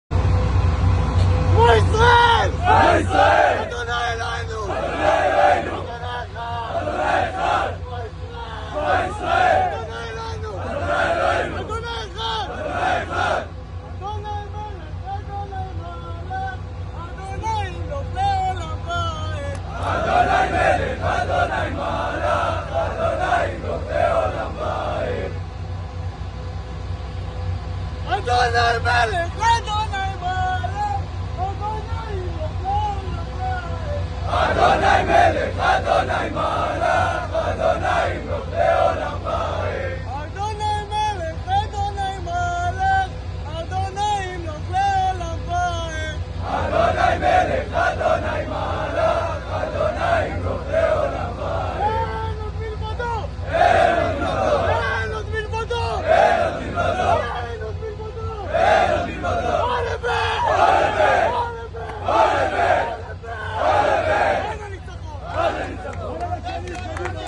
Israeli Soldiers Declaring the Faith in God and Accepting the Yoke of Heaven before Battle
Video from the IDF